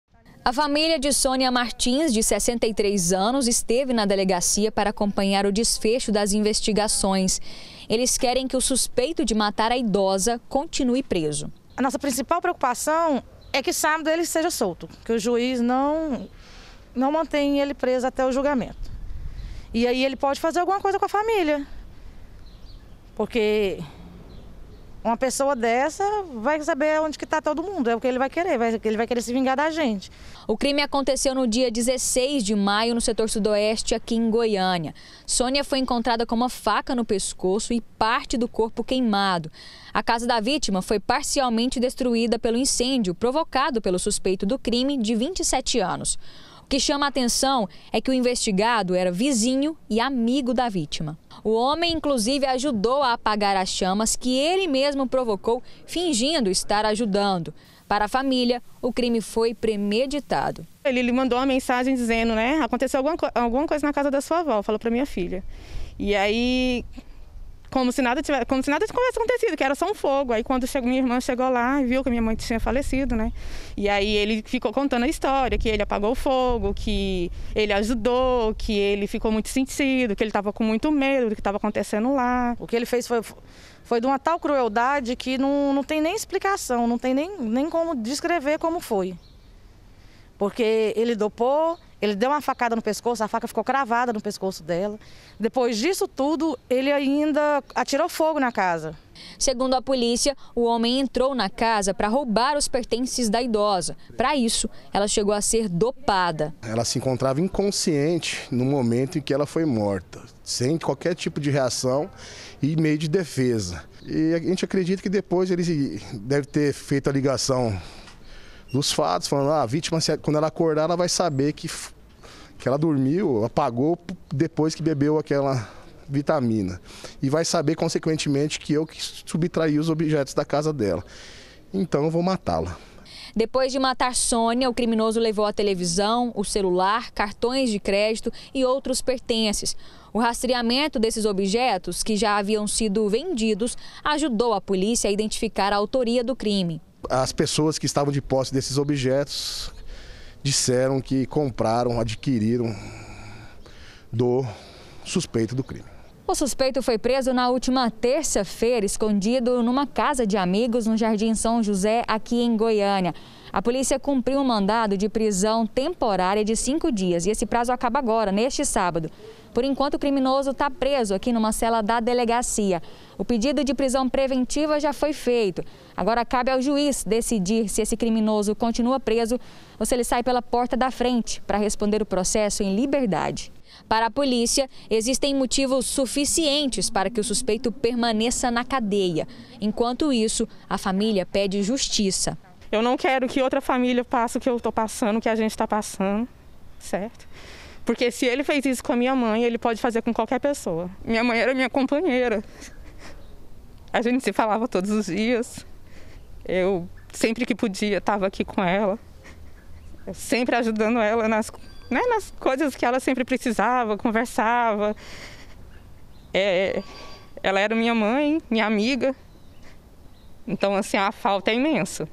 LATROCÍNIO: PRESO SUSPEITO DE MATAR IDOSA PARA ROUBAR
HOMEM, DE 27 ANOS, É PRESO SUSPEITO DE MATAR IDOSA DURANTE ASSALTO, EM MAIO DESSE ANO. O CRIME ACONTECEU NO SETOR SUDOESTE.